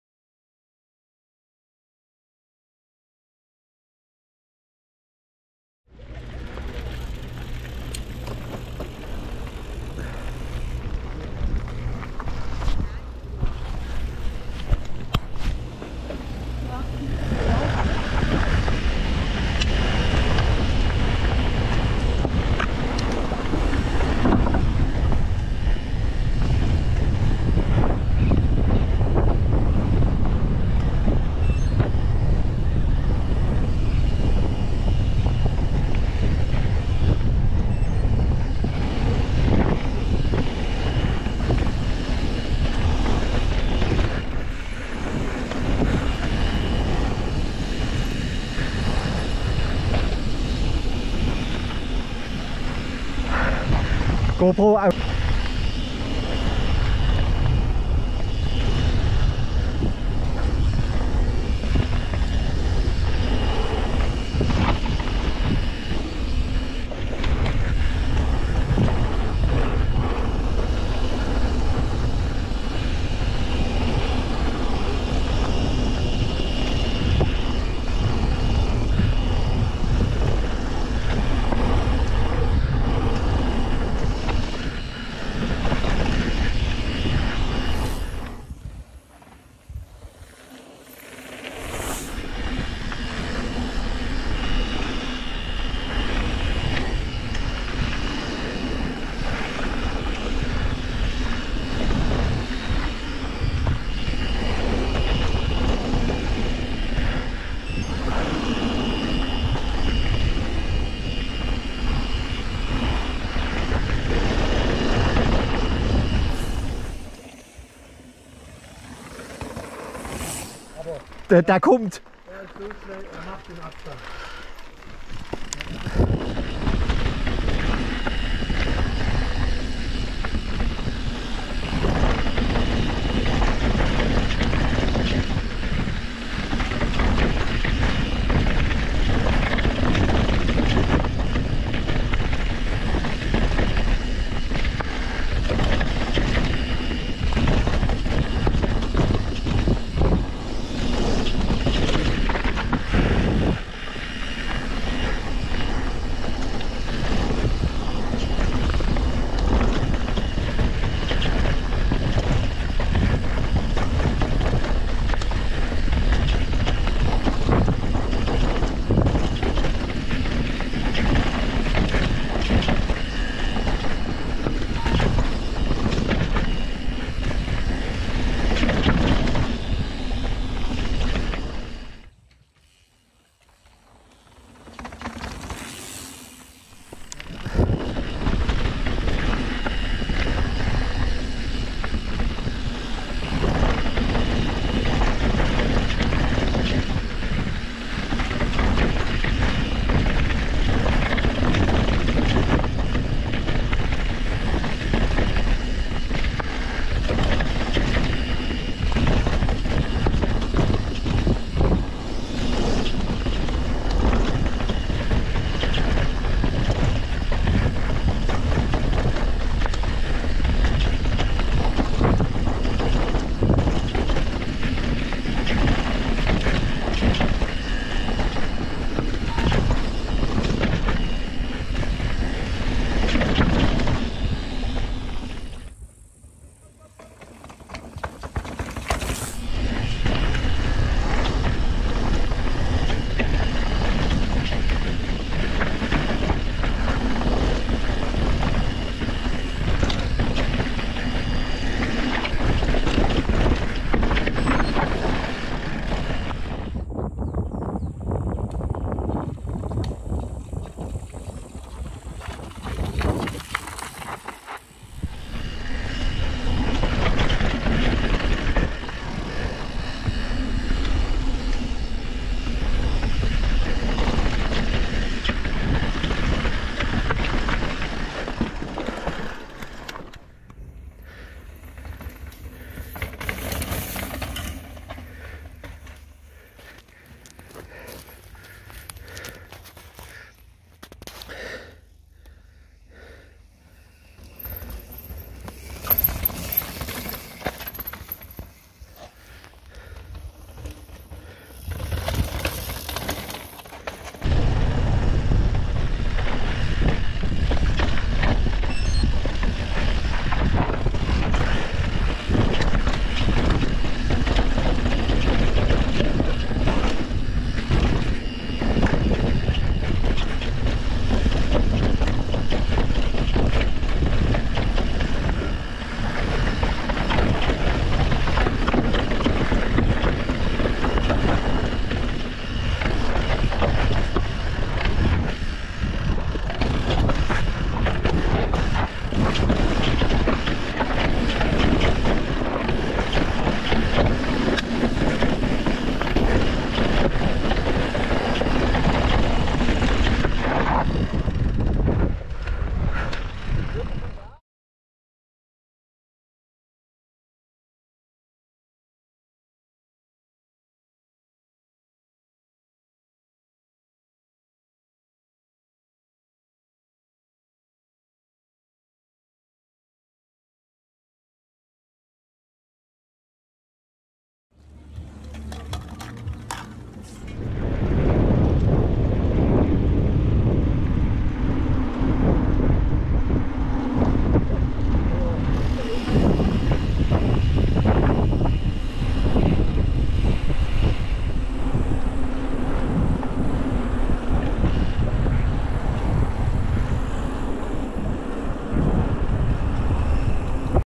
Video 3 Länder Enduro Schöneben
Reschensee
Im Anschluss an den Bunkertrail
zum oberen Schönebnen Flowtrail, und
dem unteren Enduro Trail  ;-)